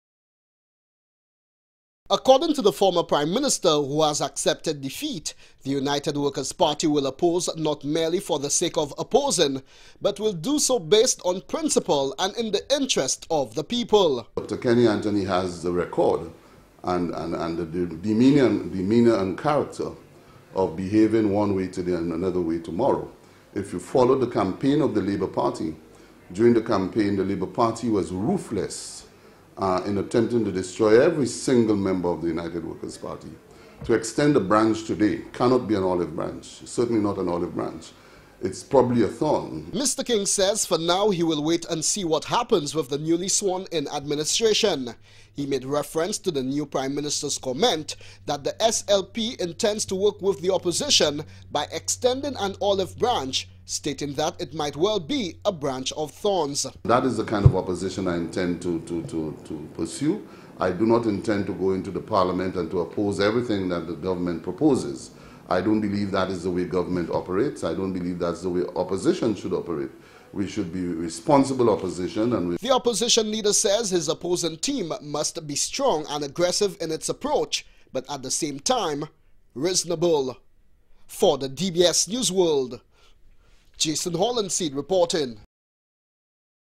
ALL THE KINGS MEN
The Political Leader of the United Workers Party is promising a very vibrant opposition.
Stephenson King says his team will be very vigilant towards the St. Lucia Labour Party administration.